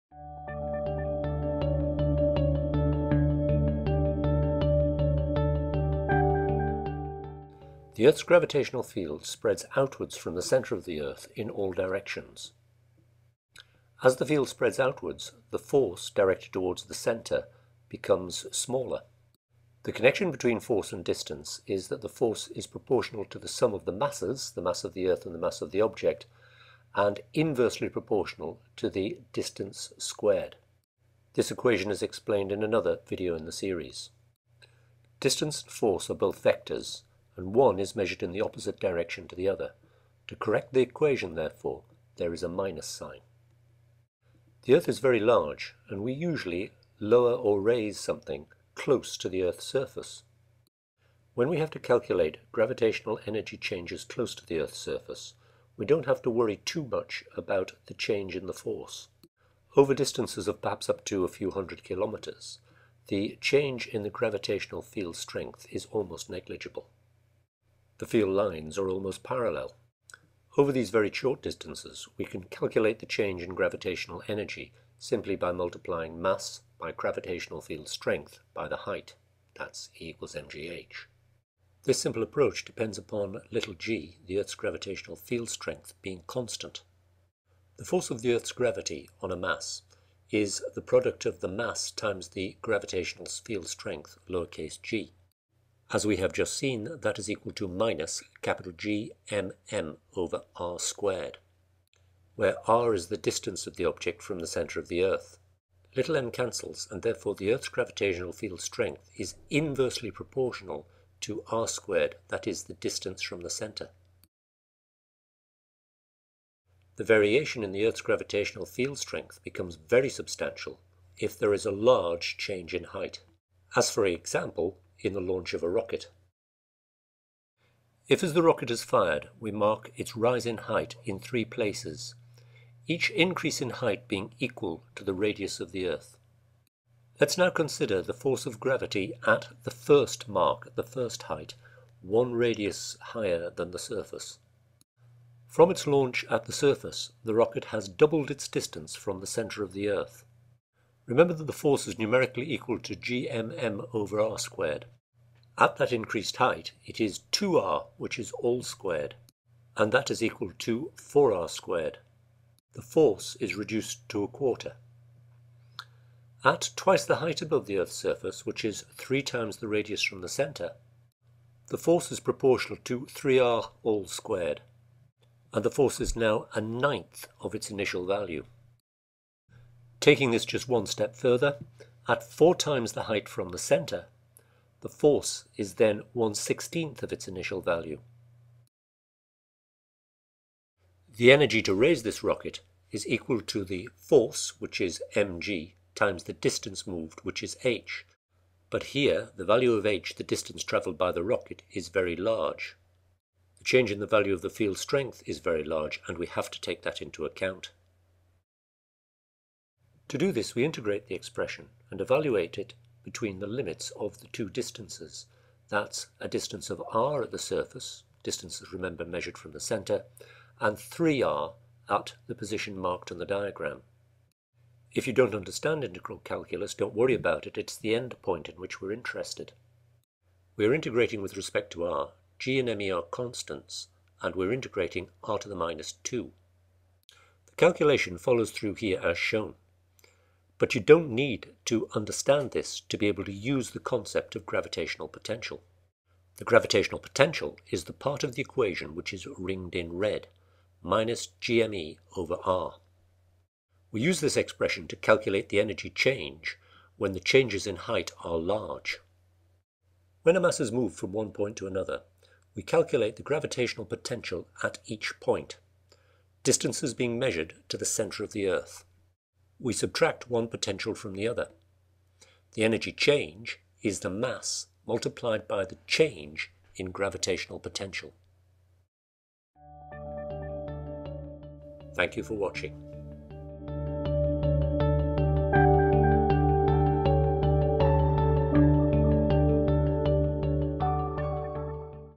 Gravitational potential and gravitational potential energy: from fizzics.org
If you need to learn or revise Newton's Law of Gravity there is a video lesson
Gravitational potential and gravitational potential energy. are ideas used to calculate the forces and energy required to move an object within a gravitational field.
When we calculate changes in gravitational potential energy over short distances near to the earth we use the simple equation “F = mgh”. However over larger distances the value of g varies. The video explains why and how changes in the gravitational field strength can be taken into account.  Notes and many more video lessons available